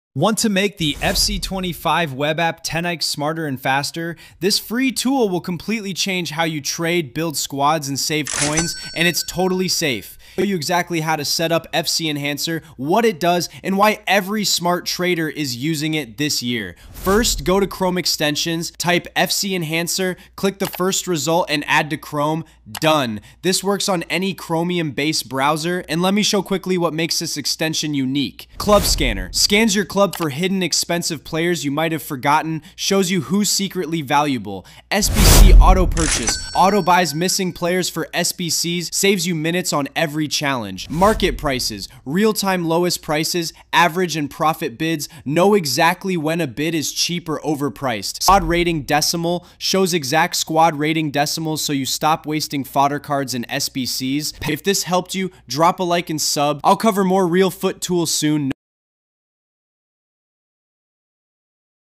The Ultimate Web app Tool You should not miss EA FC26 (FC Enhancer) !
CHECK THE FULL VIDEO IN MY CHANNEL !! EXTENSION LINK THERE
🎮 FC 26 WebApp Just Got Smarter!
In this video, I show you how to set up and use the FREE Chrome extension FC Enhancer to make your EA FC 26 Ultimate Team WebApp experience 10x faster, smarter, and more profitable.

✅ Auto-complete SBCs
✅ Scan your club for hidden high-value players
✅ See exact real-time market prices
✅ Find easy profit bids (undervalued cards)
✅ Use a free sniping tool
✅ View squad rating decimals (no wasted fodder!)
✅ Get real pack odds and community reviews
✅ Auto-pick and skip annoying popups